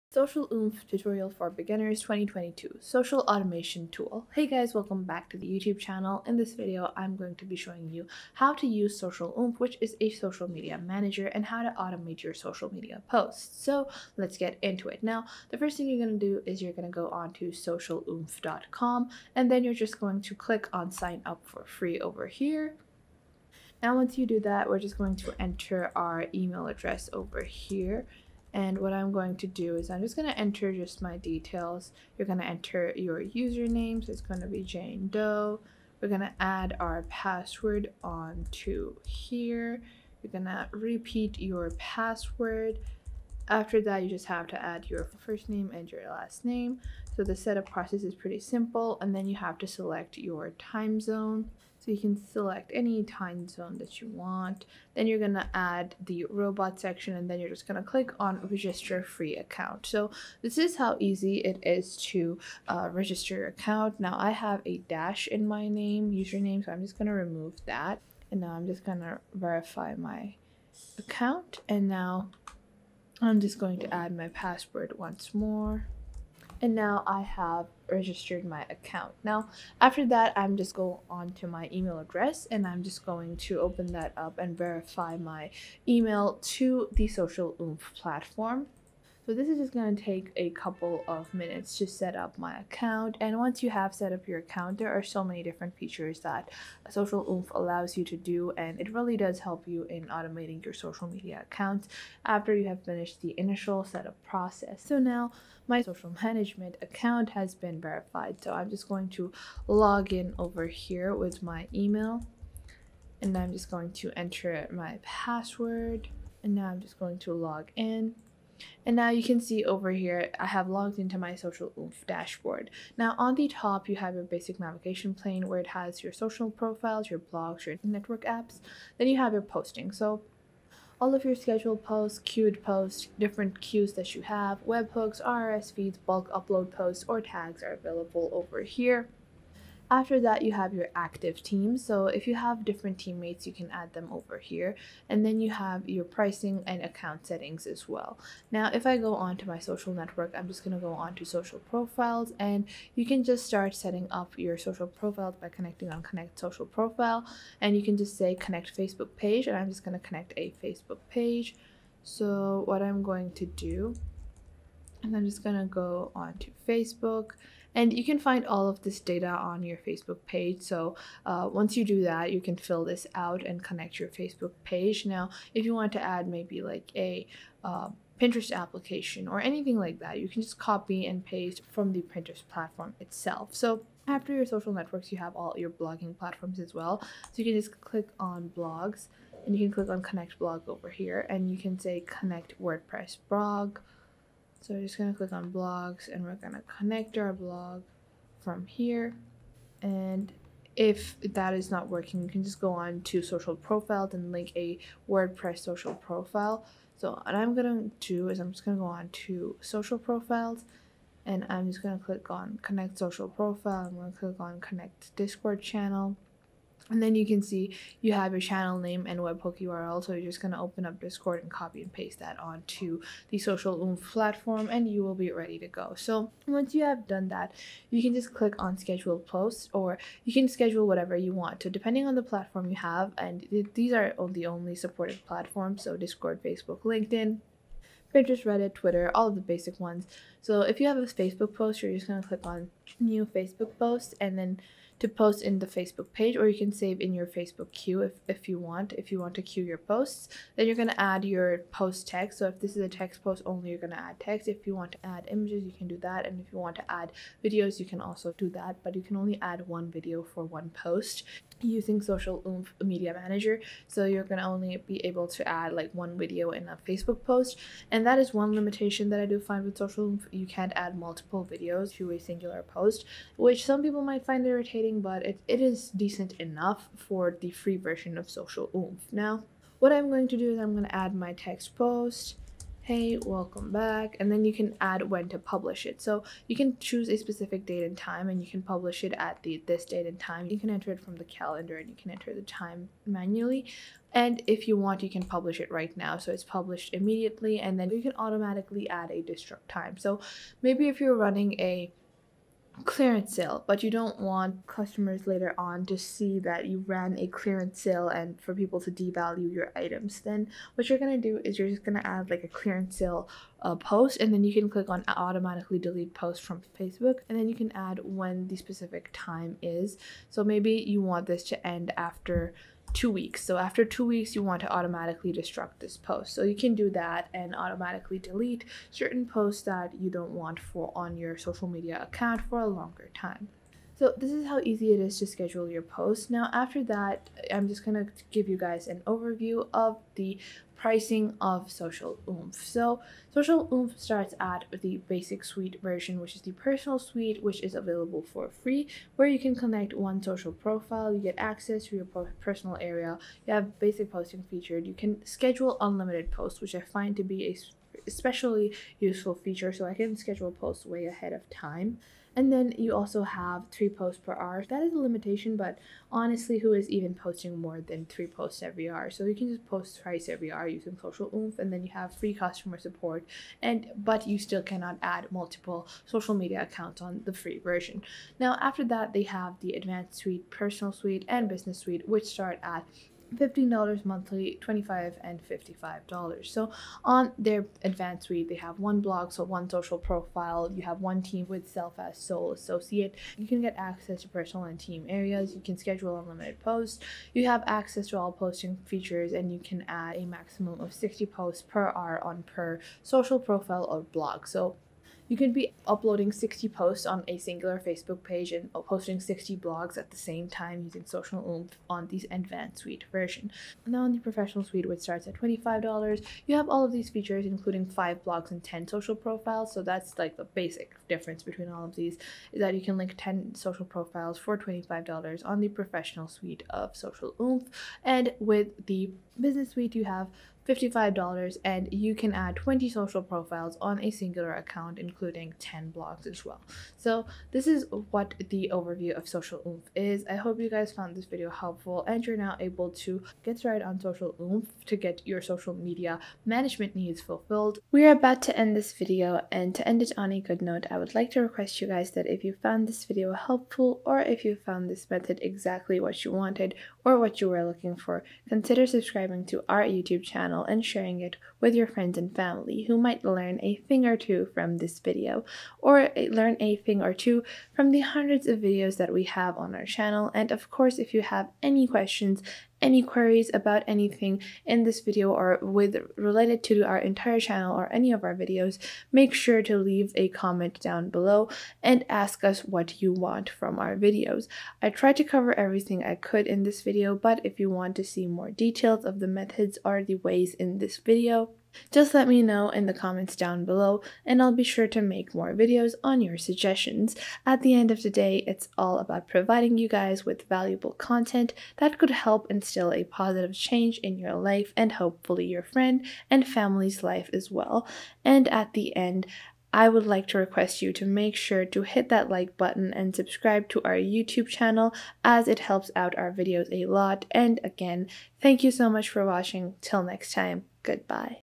Social Oomph Tutorial For Beginners 2025 (Social Media Automation Tool)
In this video I show you  How you can Use SocialOomph and How it is better for you to use for your Social Media Management in 2025 and what are the different features that Socialoomph.com offers to help you with your ultimate productivity and management needs.